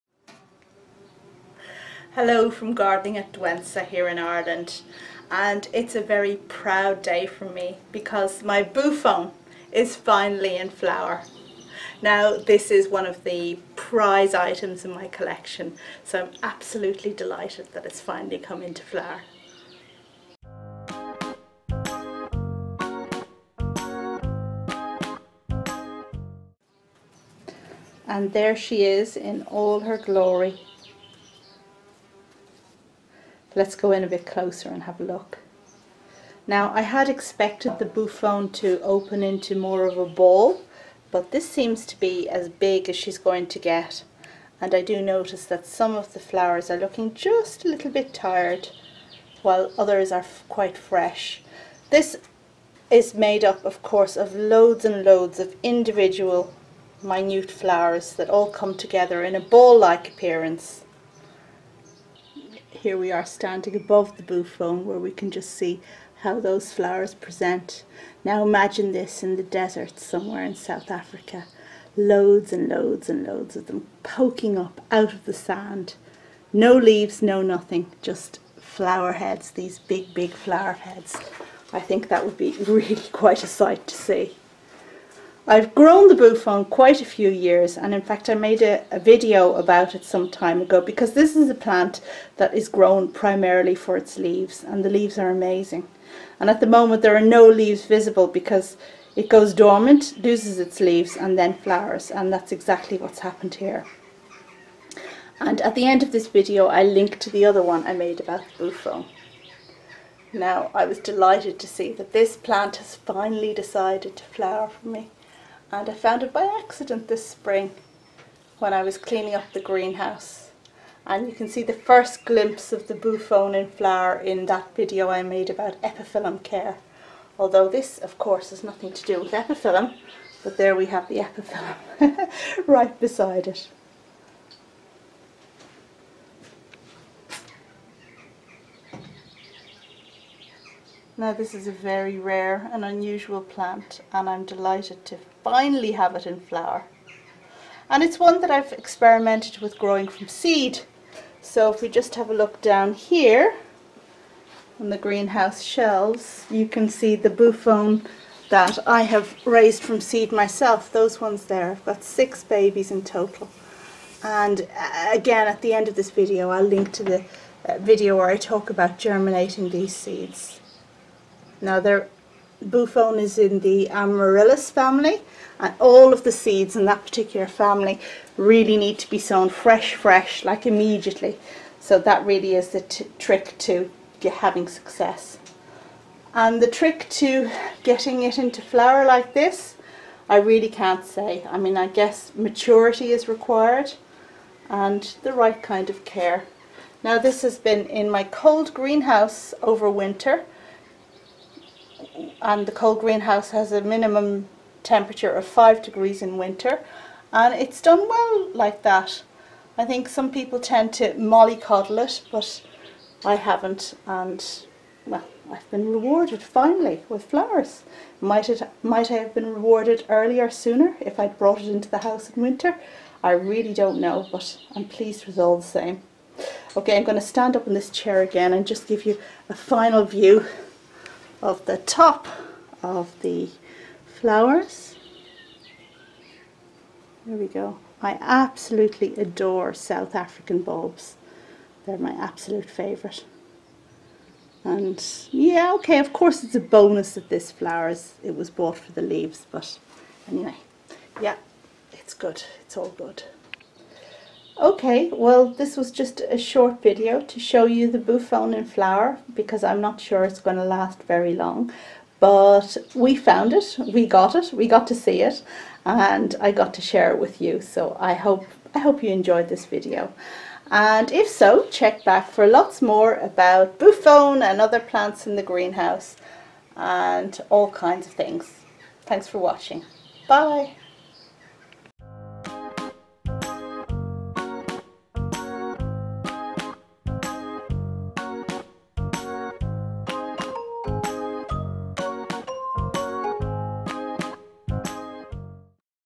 1st Flowering of Rare SA Bulb, Boophone disticha
This is the first flowering or my rare South African bulb, Boophone disticha

I invite you to join in our collaborative growing. Grow a plant to remember my son, Josh!